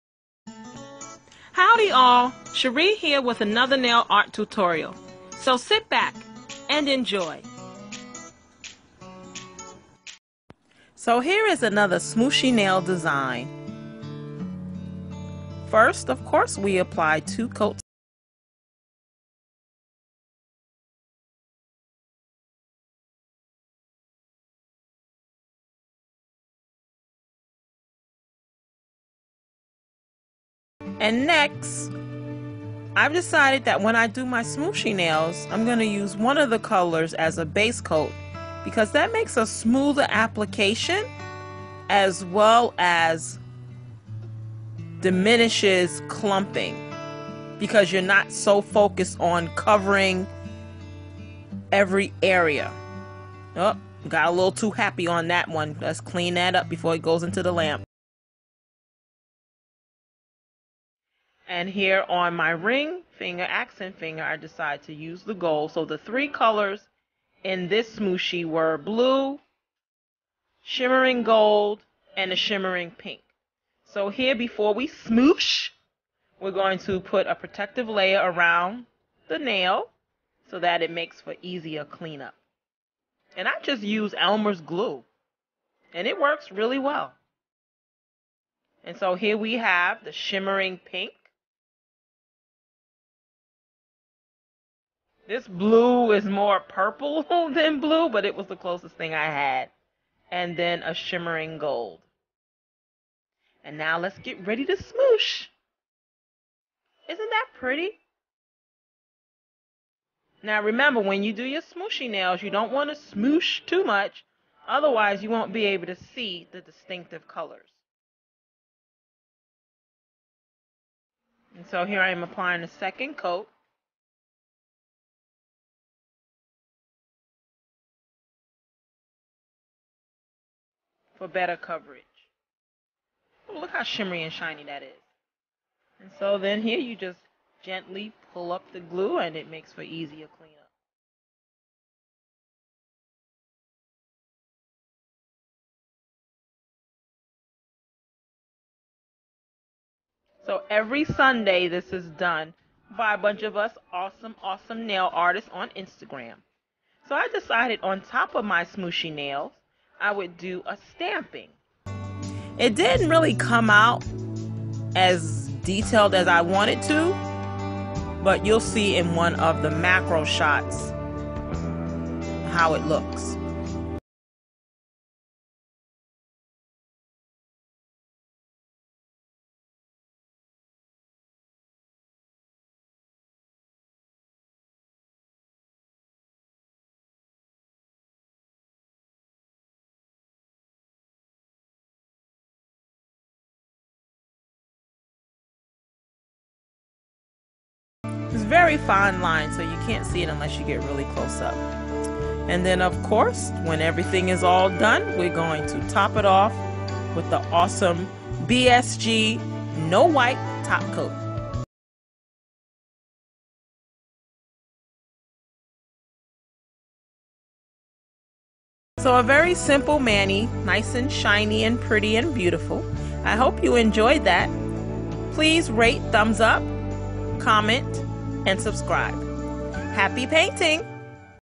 Smooshy Nail Sunday Shimmer
This was a trend for a while on IG. The group would pick 2-4 colors and then you smoosh all the colors together for a cool marble design and then most of us would add some kind of art on top of the colors.